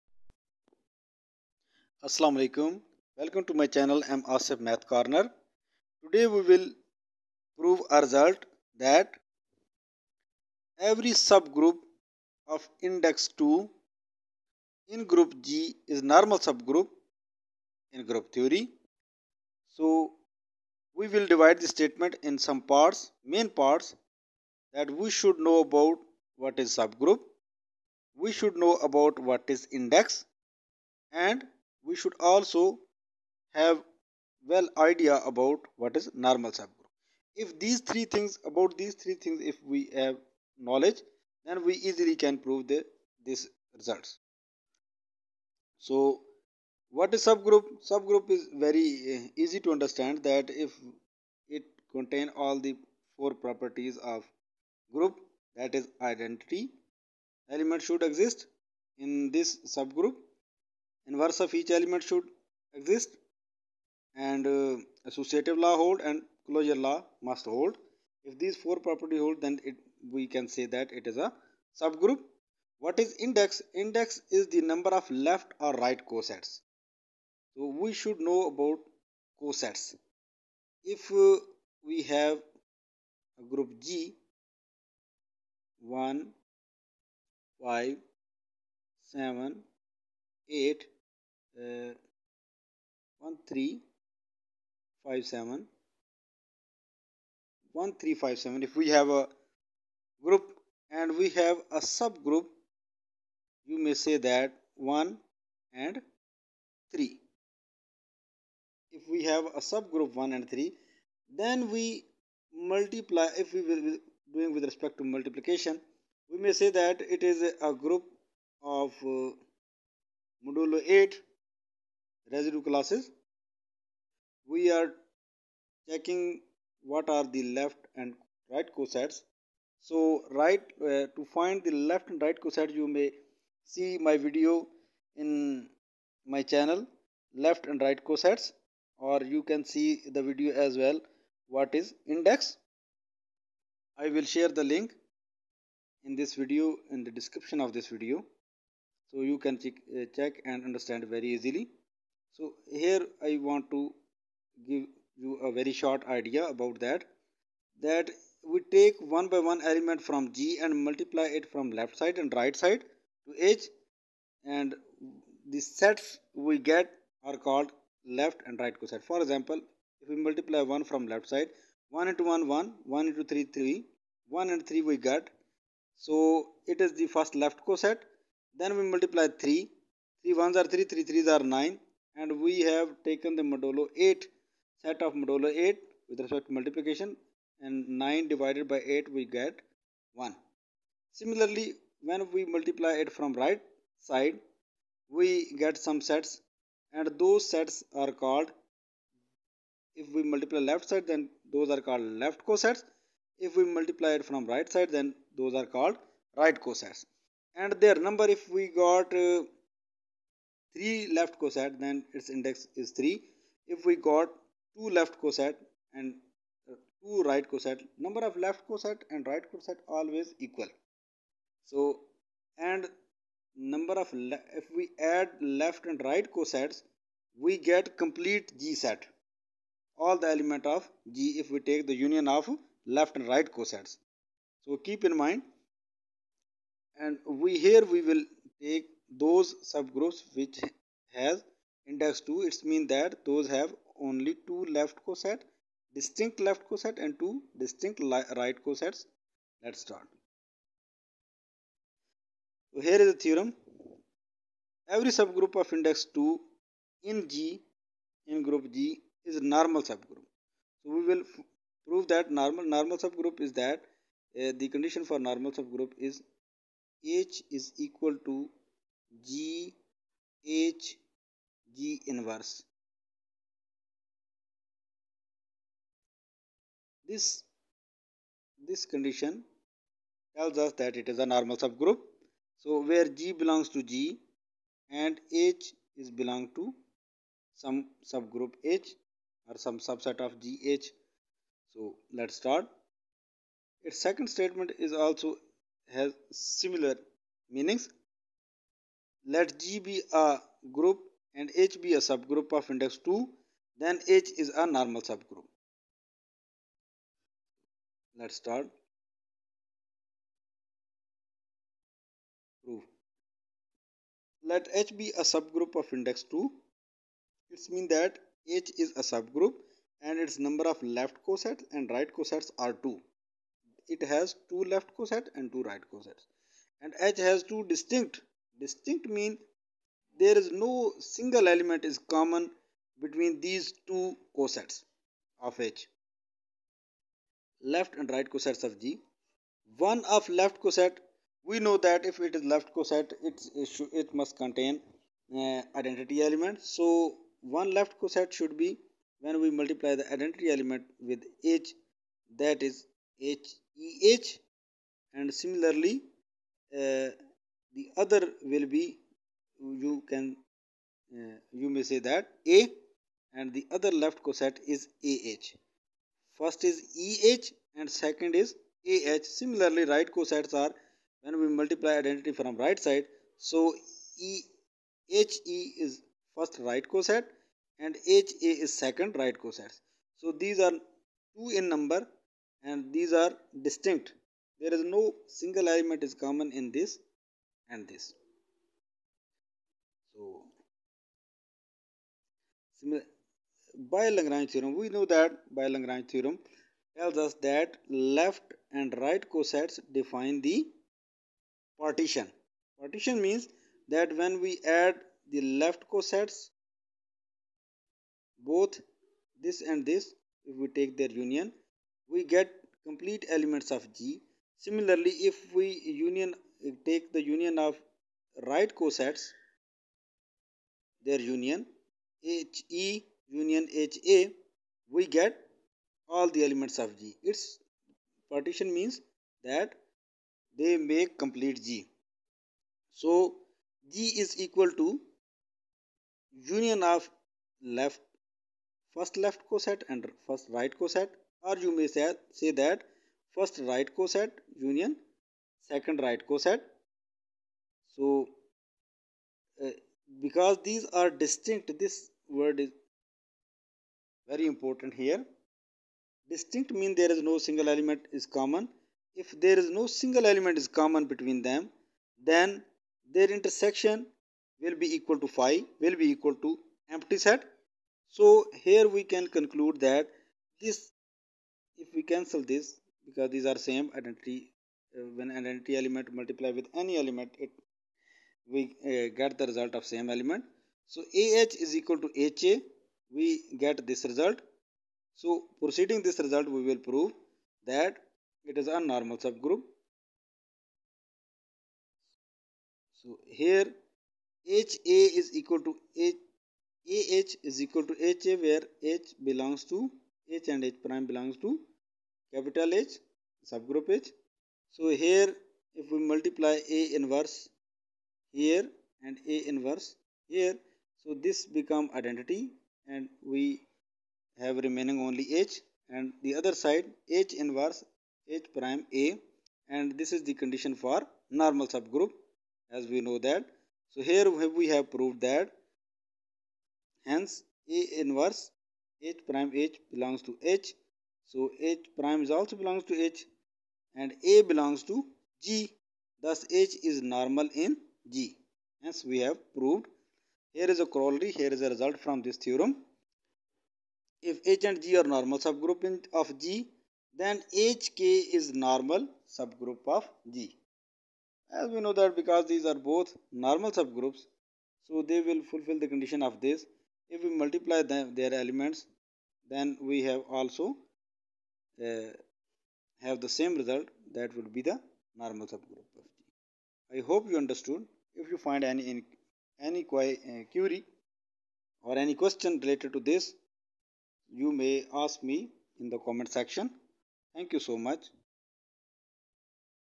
Every SubGroup of Index 2 In a Group G is Normal SubGroup|Group Theory|M.Asif's Math Corner
In this video I described Every SubGroup of Index 2 In a Group G is Normal SubGroup

To understand this Result
we need know about these topics

M.Asif's Math Corner